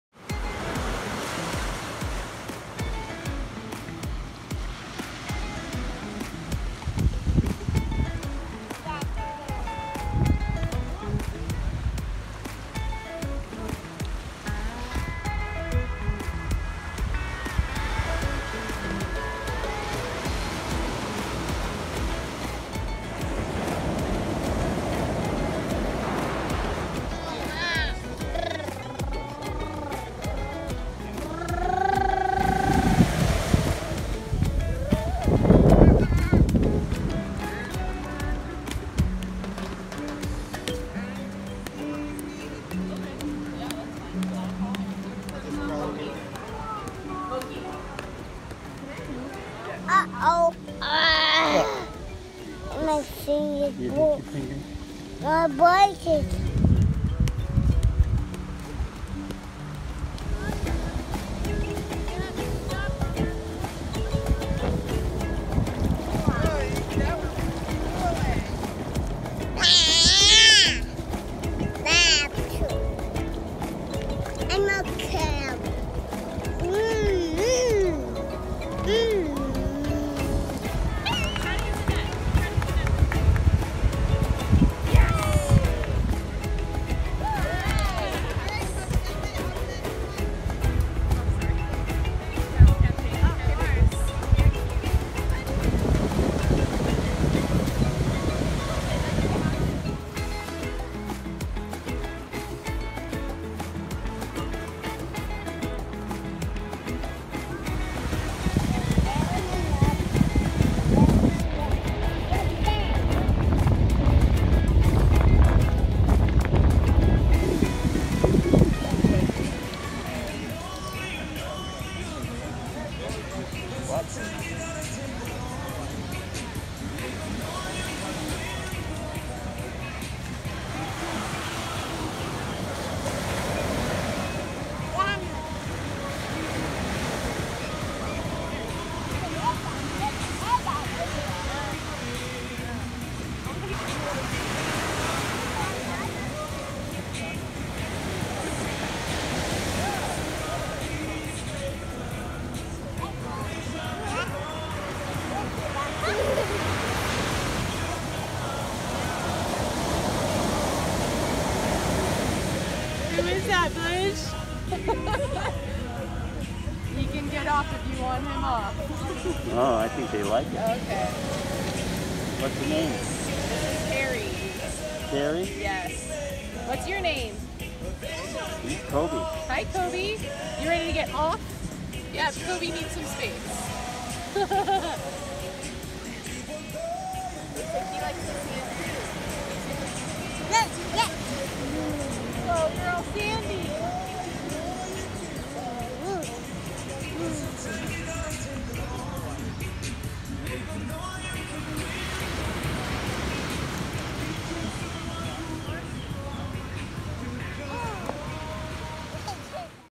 Maui Beaches • Kobe West World • Jan 2021
Kobe West 27 Months: Playing in Sand and Water on Beaches Around Maui.